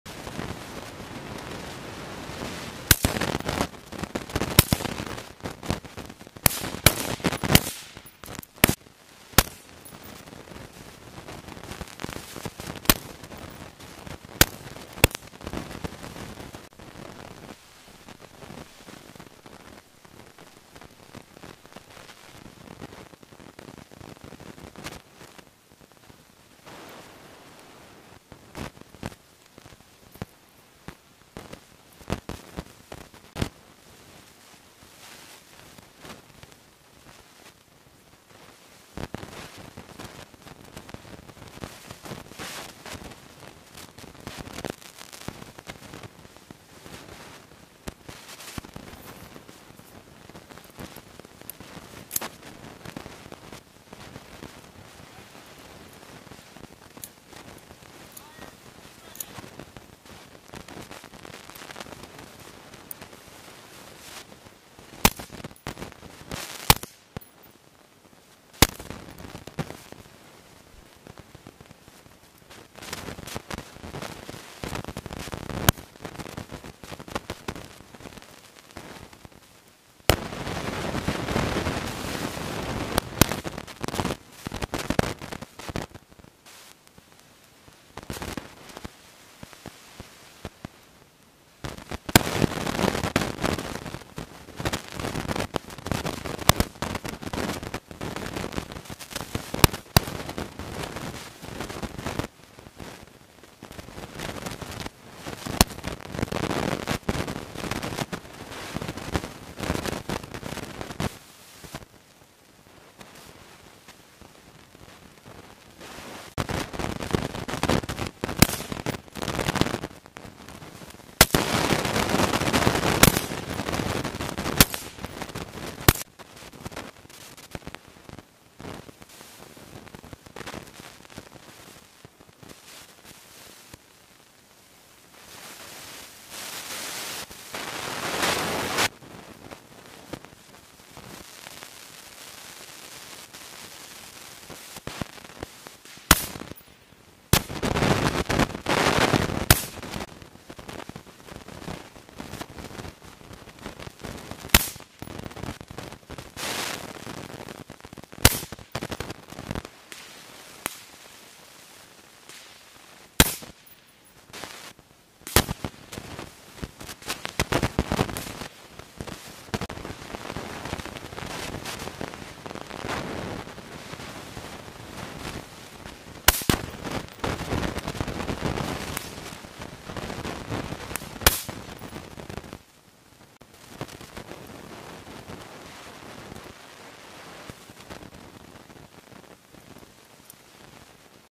Soldiers Zero and fire M4 Carbines at USAREUR-AF Best Squad Competition
U.S. Soldiers assigned to U.S. Army 56th Artillery Command team zero their M4's during the U.S. Army Europe and Africa Best Squad Competition at Grafenwoehr Training Area, Germany, Aug. 10, 2022.

BY, GERMANY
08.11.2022

Teams across U.S. Army Europe and Africa test their tactical proficiency, communication, and overall cohesion as they compete for the title of Best Squad. Winners of this competition will advance to represent U.S. Army Europe and Africa at the U.S. Army Best Squad Competition at Fort Bragg, North Carolina later this year. (U.S. Army video by Kevin Sterling Payne)

Film Credits: Video by Kevin Payne